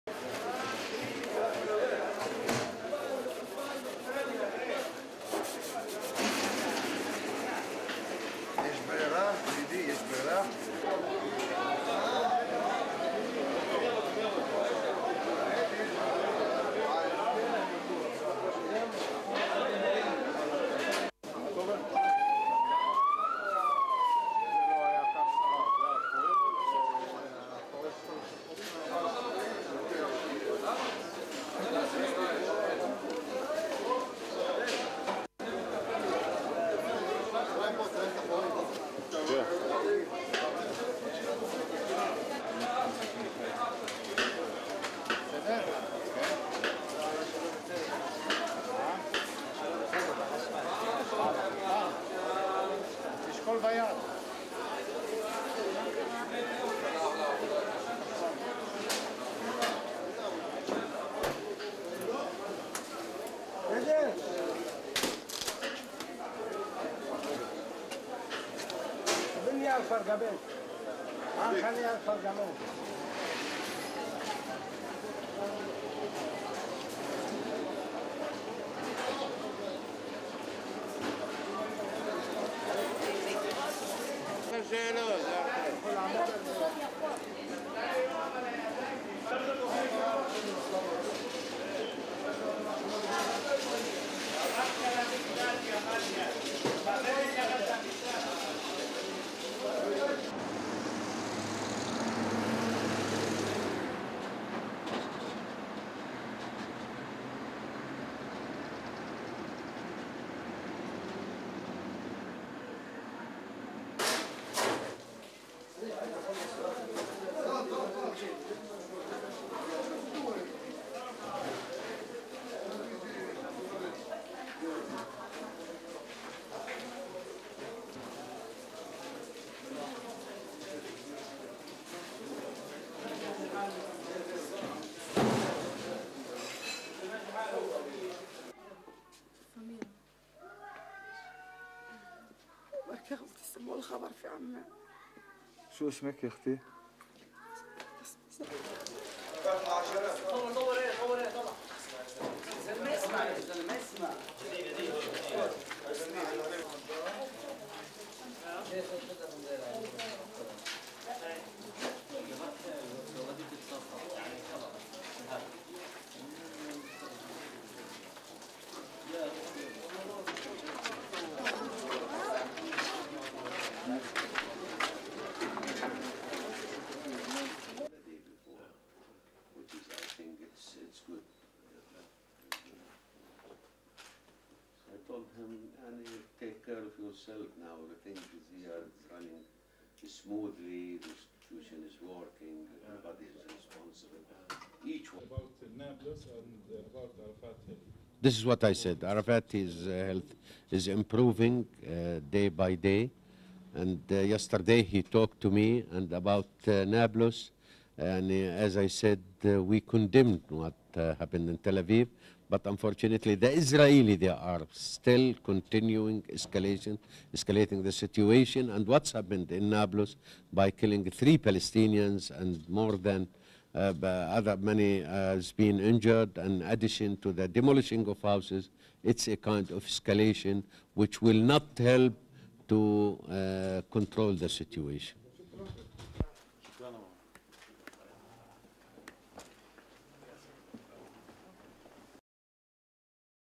Aftermath of Tel Aviv bomb, bomber's house demolished, Qureia
Ha Carmel Market, Tel Aviv, Israel
1. Stall owner cleaning market stand
2. Wreckage of blast, tilt down to site
3. Various of security guard and people at site of blast
4. Pan of people buying fruit and vegetables
5. Man lighting candles at scene of blast
6. Man selling fruit
7. Man buying fruit
8. Market stalls

Al-Askar refugee camp, Nablus, West Bank
++Night shots++
9. Convoy of Israeli military vehicles leaving house
10. Various of rubble inside house of bomber's family
11. Family member crying
12. Various of demolished house
13. People standing outside demolished house of bomber

Ramallah, West Bank
14. Palestinian Prime Minister Ahmed Qureia in meeting
15. SOUNDBITE: (English) Ahmed Qureia, Palestinian Prime Minister:
"This is what I said: Arafat is improving day by day and yesterday he talked to me about Nablus and as I said we condemned what happened in Tel Aviv. But unfortunately the Israelis, they are still continuing escalating the situation and what's happened in Nablus by killing the three Palestinians (three militants associated with Fatah) and more than, many has been injured and in addition to the demolishing of houses it's a kind of escalation which will not help to control the situation."
16. Qureia leaving

STORYLINE:

Israel was reacting with uncharacteristic calm on Tuesday after a suicide bombing in Tel Aviv by a Palestinian teenager killed three Israelis - a test of Israel's pledge to show restraint in the wake of Palestinian leader Yasser Arafat's illness.

The 16-year-old bomber blew himself up on Monday on a narrow street in Tel Aviv's Carmel Market, crowded with simple stalls and stands, scattering bloody vegetables and spices on the ground.

Early on Tuesday morning the Israeli army destroyed the home of the bomber and of the two men Israel says were behind the attack.

Using general terms, Israeli Prime Minister Ariel Sharon denounced the attack and pledged action.

The Tel Aviv bombing, which killed three and wounded 32, was the 117th suicide bombing since the outbreak of Israeli-Palestinian fighting in 2000 and was the first since Arafat left for France last week.

In all, 494 Israelis have been killed in the attacks.

The Popular Front for the Liberation of Palestine, a radical Palestine Liberation Organisation (PLO) faction, claimed responsibility, identifying the assailant as Eli Amer Alfar, from the Askar refugee camp near the West Bank city of Nablus.

Alfar was only 16 years old, one of the youngest Palestinian suicide bombers, and his parents lashed out at the militants who recruited him.

On Monday Palestinian Prime Minister Ahmed Qureia said that both he and Arafat "condemned what happened in Tel Aviv".

The army said that during an attempt to arrest five members of the Fatah-linked Al Aqsa Martyrs' Brigades, the wanted men drew pistols. The soldiers fired first, killing three men, wounding one and arresting a fifth, the Israeli military said.